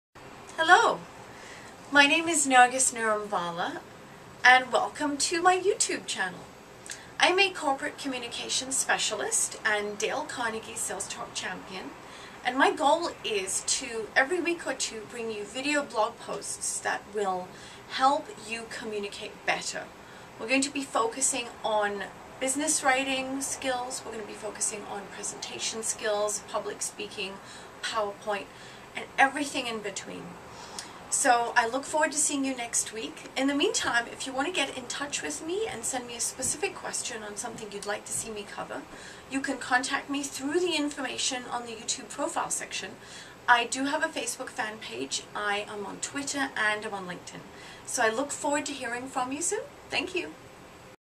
Narges Nirumvala: WELCOME To My YouTube Channel!
Narges Nirumvala is a Corporate Communication Specialist, writer, trainer, professional speaker and Dale Carnegie Sales Talk Champion. for more information.